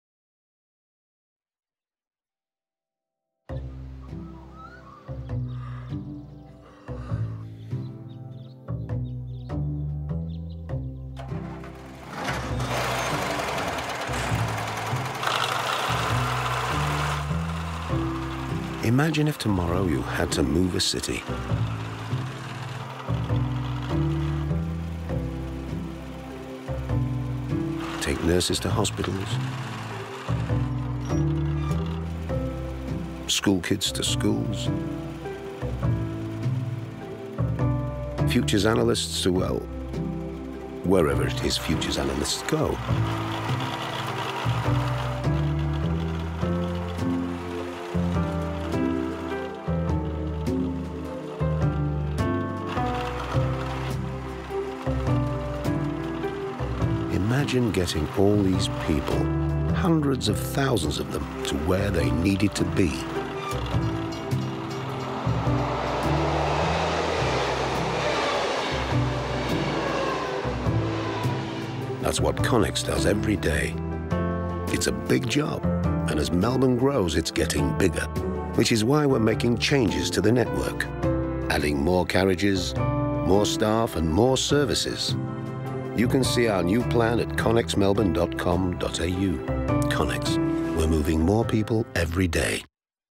Connex TVC from Leo Burnett Melbourne
Train operator Connex promotes changes to its operations in a TVC created by Leo Burnett Melbourne